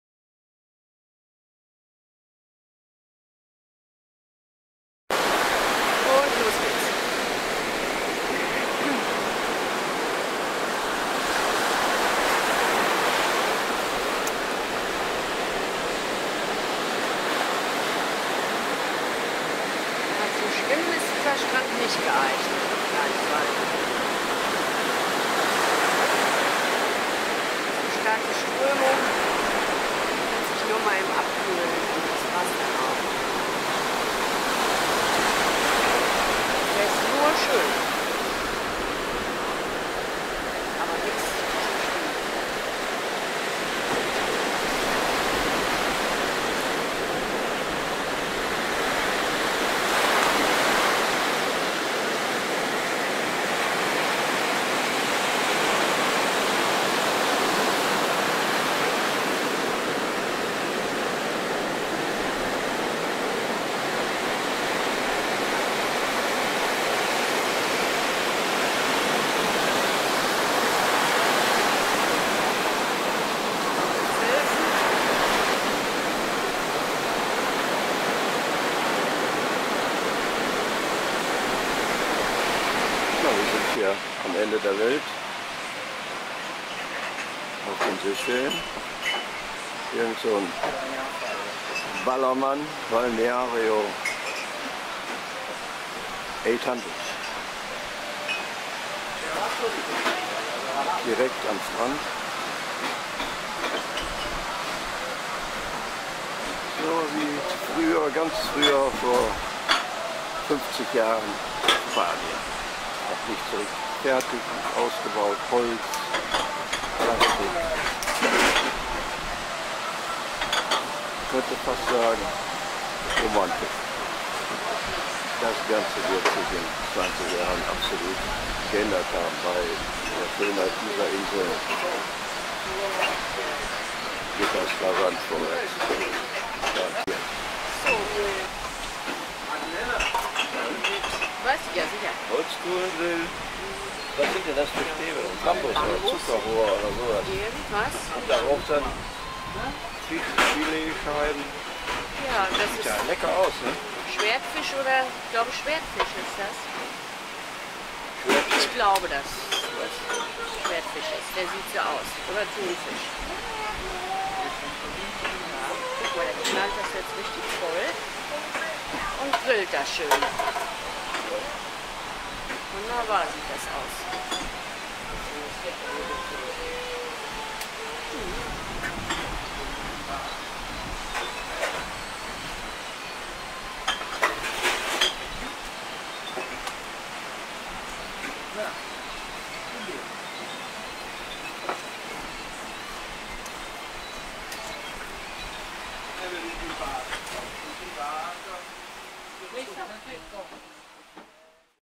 Urlaub Seychellen - La Digue: Wilder Strand und wilde Wellen
Original-Ton West. Sehr ursprünglicher Strand mit Holzbretter Restaurant.
Mal in eigener Sache:  Wir sind nicht daran interessiert, die Seychellen als solches in Szene zu setzen ! Da gibt es professionelle Videos ohne Ende. Unsere Videos sind "just for fun"  für uns und andere Touristen.